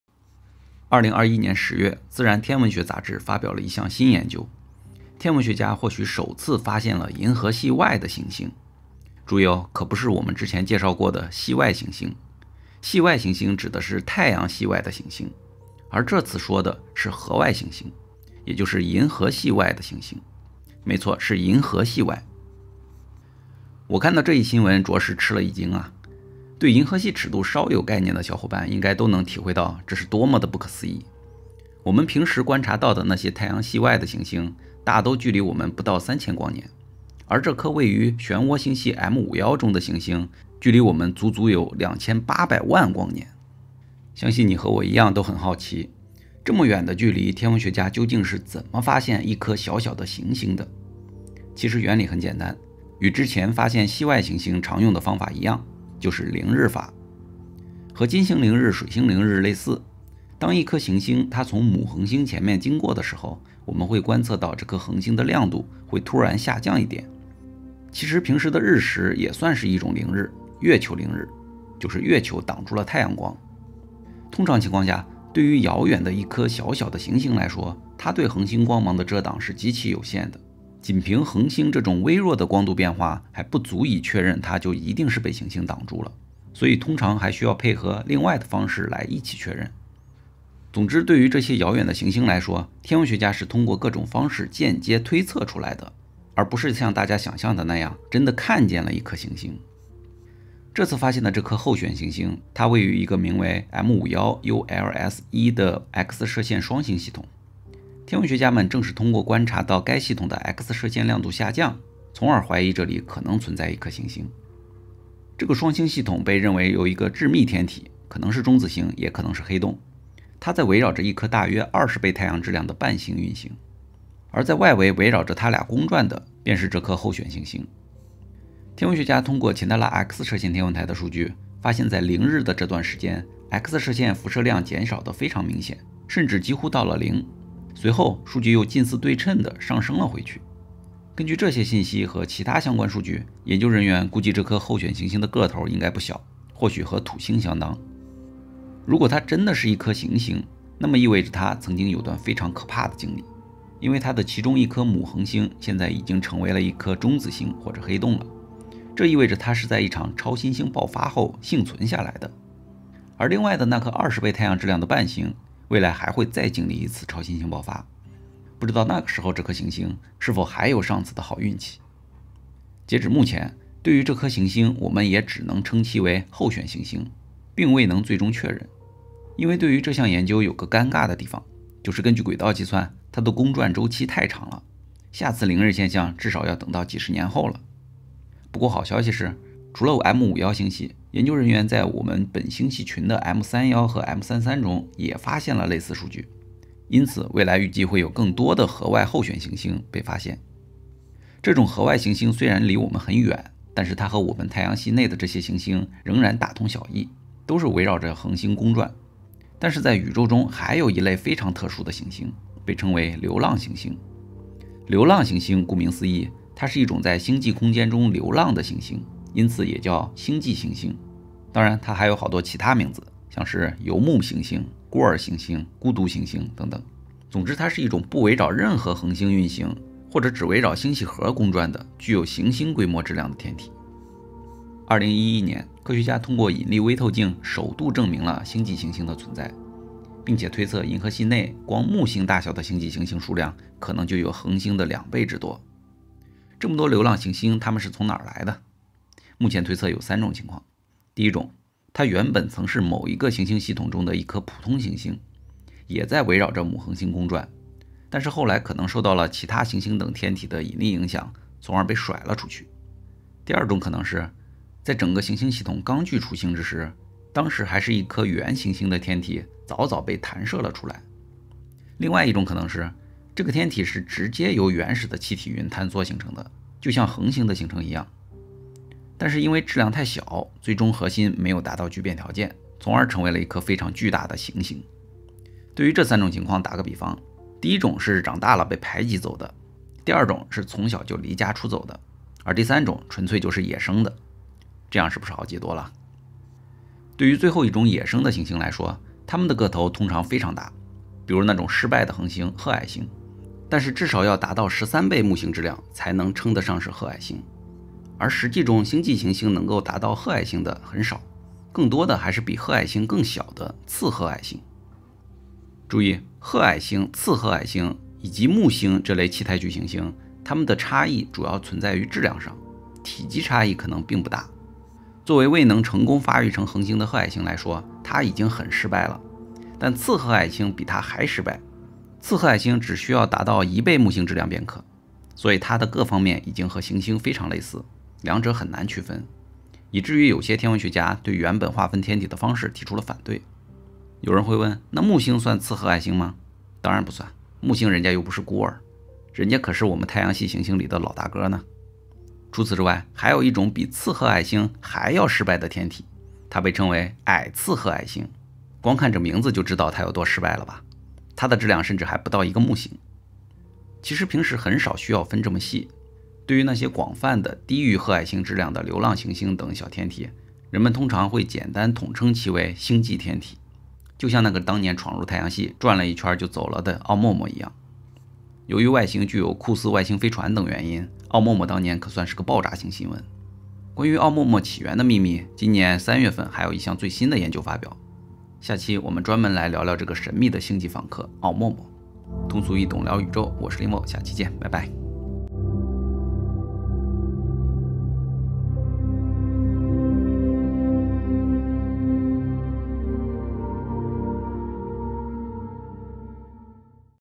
【宇宙雜談】首次發現銀河系外行星！什麽是「流浪行星」？| Linvo說宇宙
2021年10月，《自然天文學》雜誌發表了一項新研究：天文學家或許首次發現了銀河系外的行星！
註意哦，可不是太陽系外的「系外行星」，而是銀河系外的「河外行星」！

大家好，這裡是Linvo說宇宙。
希望大家在觀看我的視頻過程中學到更多宇宙相關的科學知識。